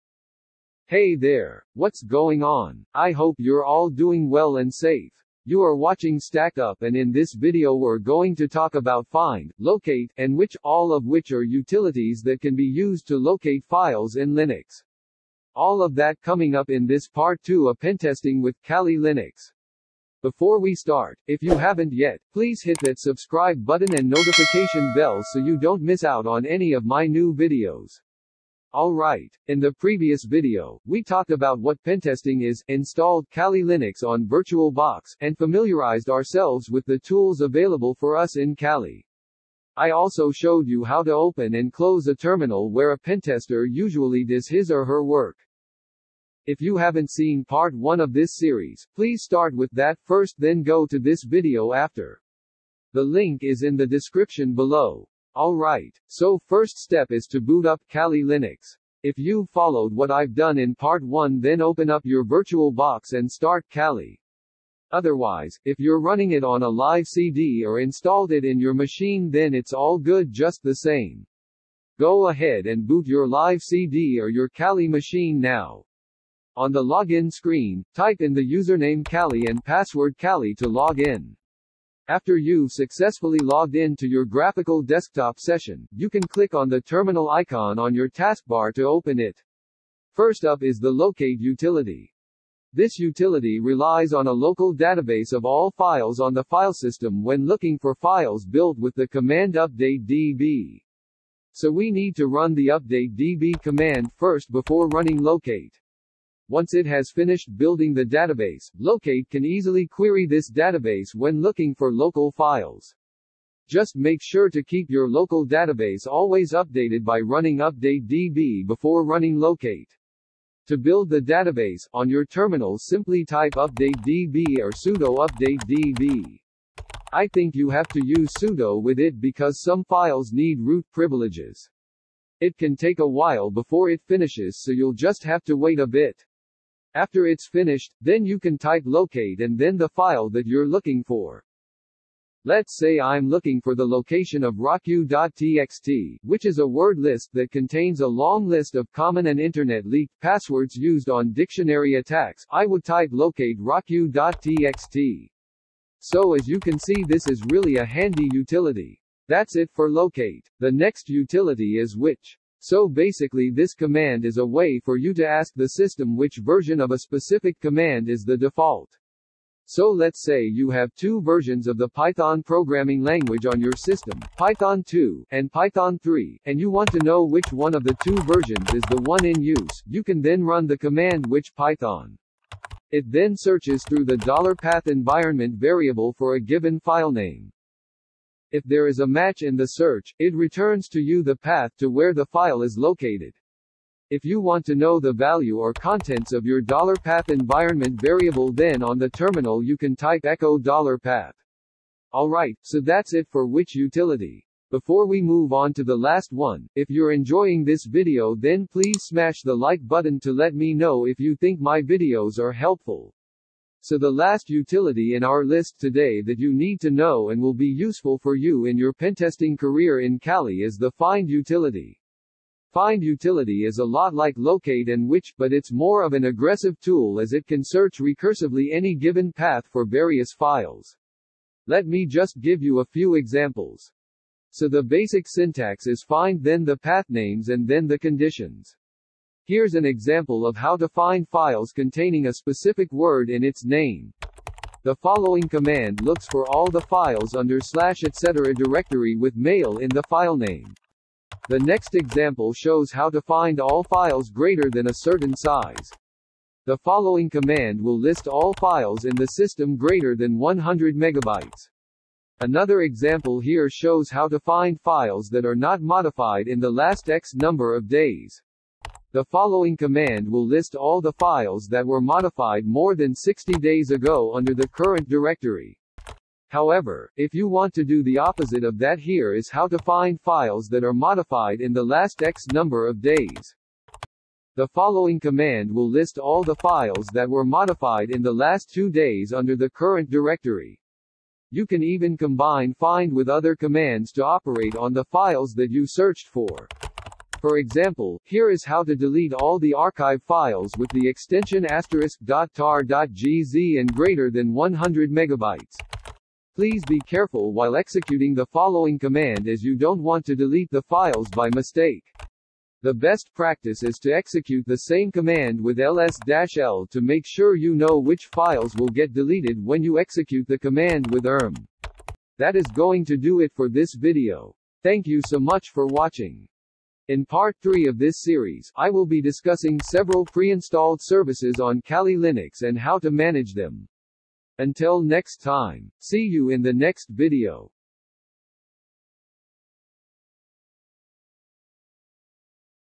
Pentesting with Kali Linux - Part 2: locate, which, and find utilities
This is part 2 of a video series in the making about Pentesting with Kali Linux.